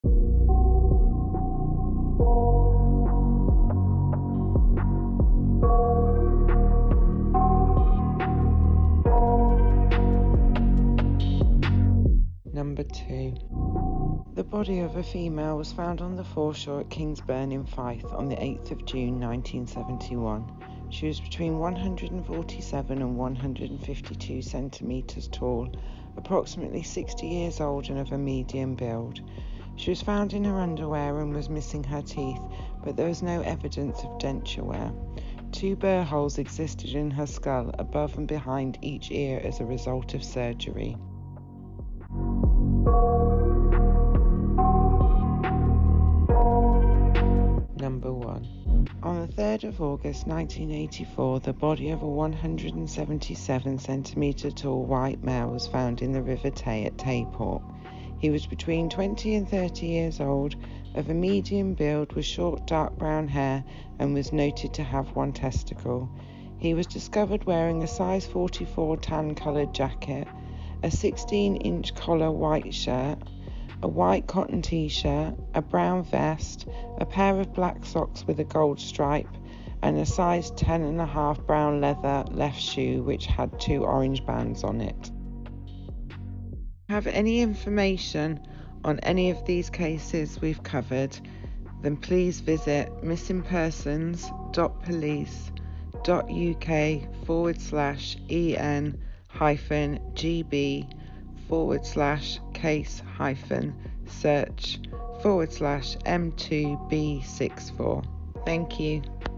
Unsolved Missing Person Cases - Fife Constabulary
Here are some of the missing persons cases which are still unsolved within the Fife Constabulary.

If you have any information on the cases covered, please